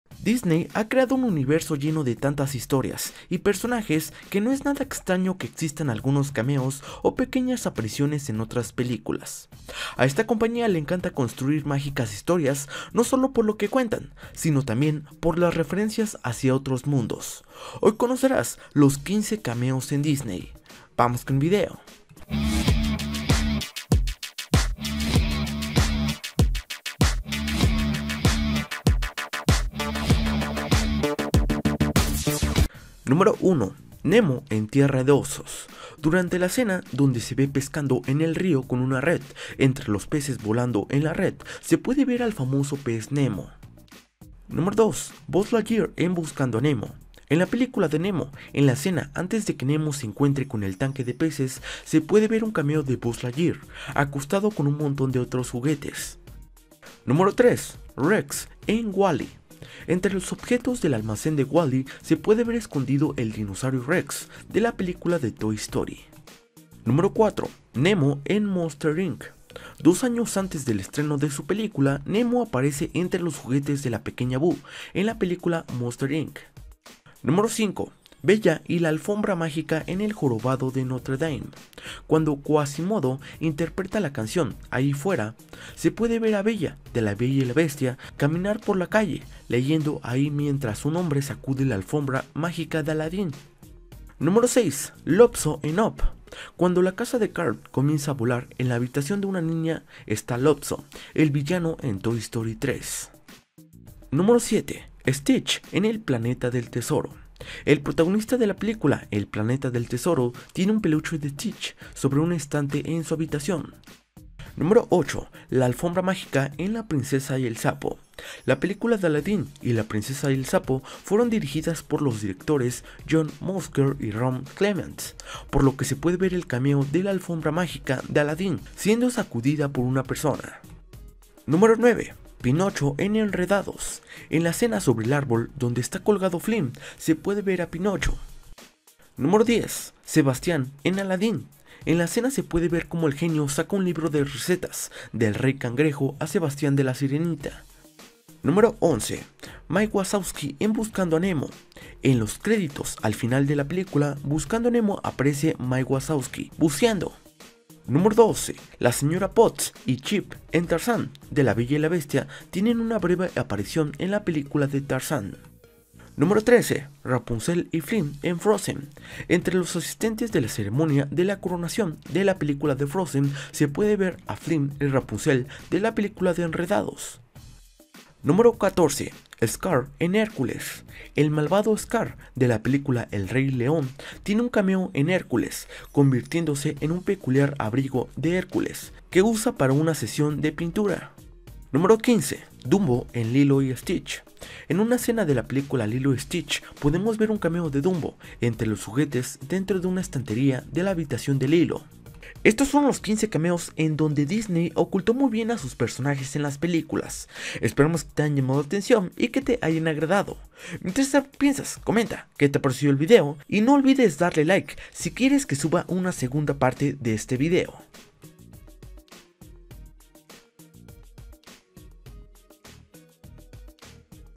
15 Increíbles Cameos en Peliculas de Disney Pixar
Disney ha creado un universo lleno de tantas historias y personajes que no es nada extraño que existan algunos cameos o pequeñas apariciones en otras películas.
A esta compañía le encanta construir mágicas historias no sólo por lo que cuentan, sino también por las referencias hacia otros mundos; Hoy Conocerás Los 15 cameos de Disney.